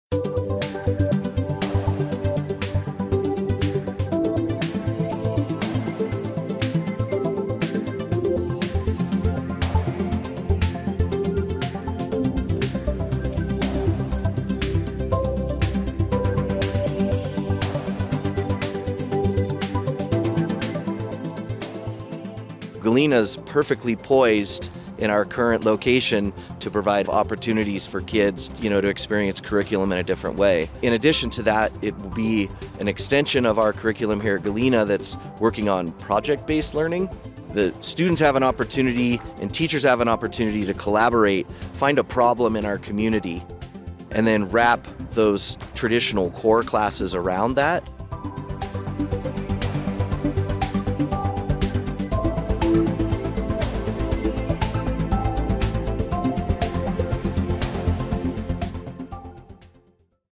Introducing the Galena Outdoor Academy!
The Outdoor Academy is a partnership between Galena High School and Sky Tavern. Outdoor Academy students will have the opportunity to learn by actively engaging in real-world and personally meaningful projects, focused on our outdoor mountain and desert resources. The Academy begins Fall 2024!